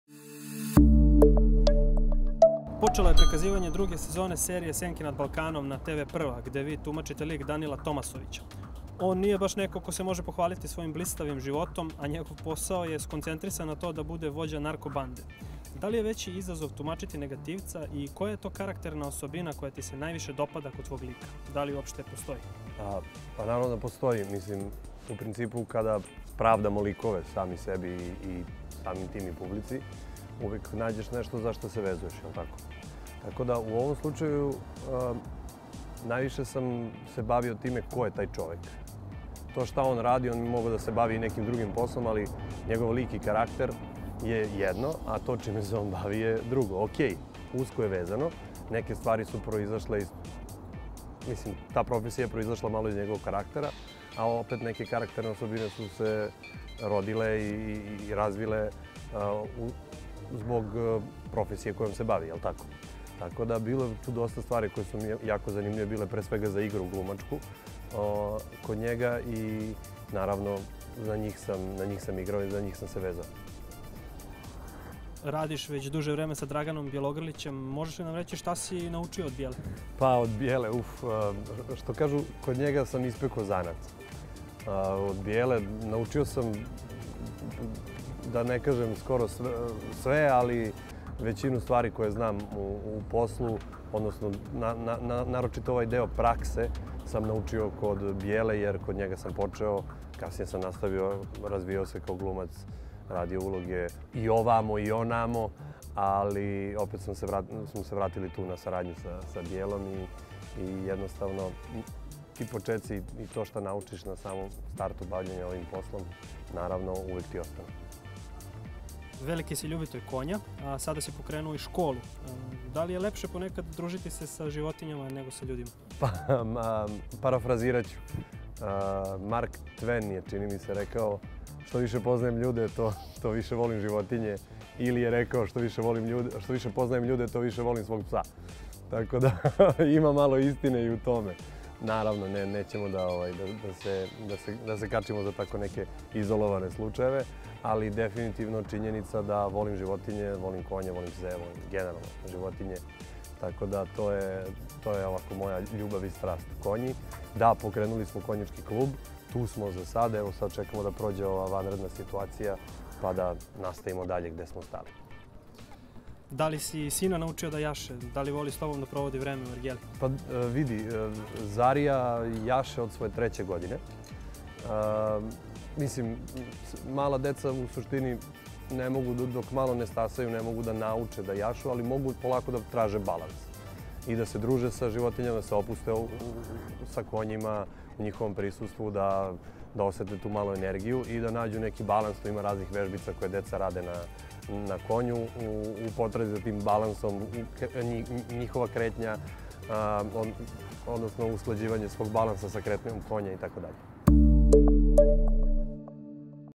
Petar Strugar otvorio dušu o velikoj ljubavi, sinu i Draganu Bjelogrliću
Osim glume, Strugaru je velika ljubav priroda, ali i konji. Za njih se vezao i svaki slobodan trenutak, osim sa sinom, voli da provede u konjičkom klubu.